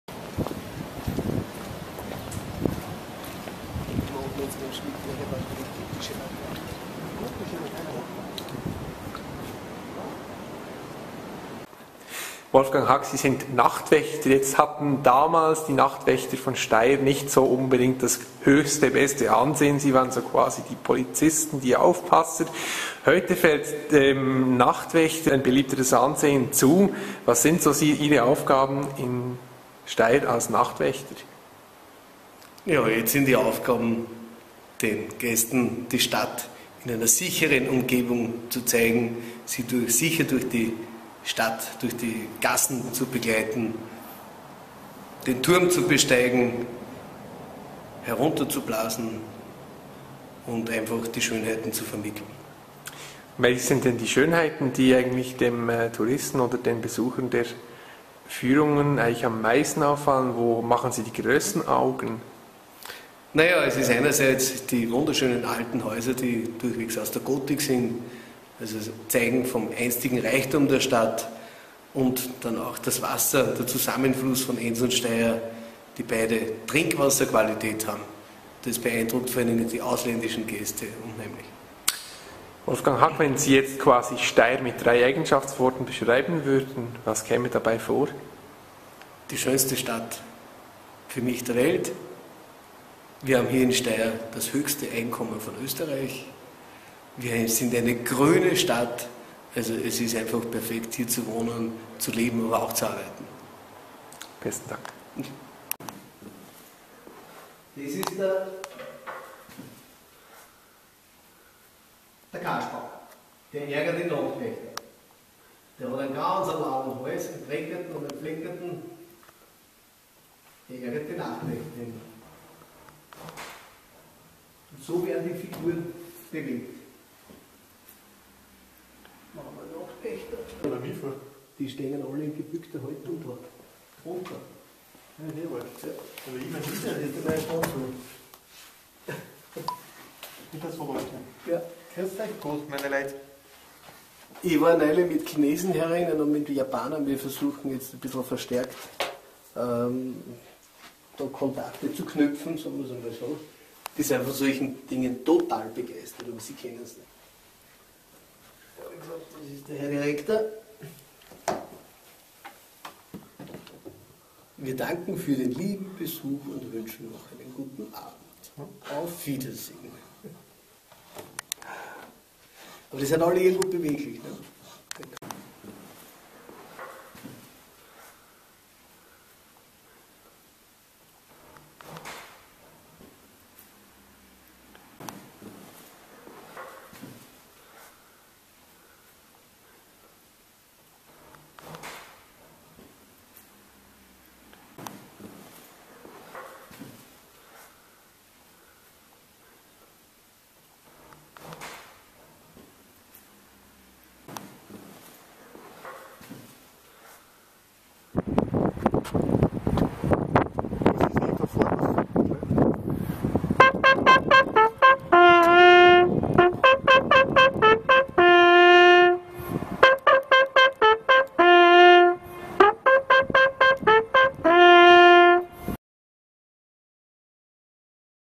Der Nachtwächter von Steyr
Wolfgang Hackl ist Nachtwächter in Steyr. Seine Familie ist seit 600 Jahren in Steyr zuhause. Hackl erzählt über die früheren und heutigen Pflichten der Nachtwächter.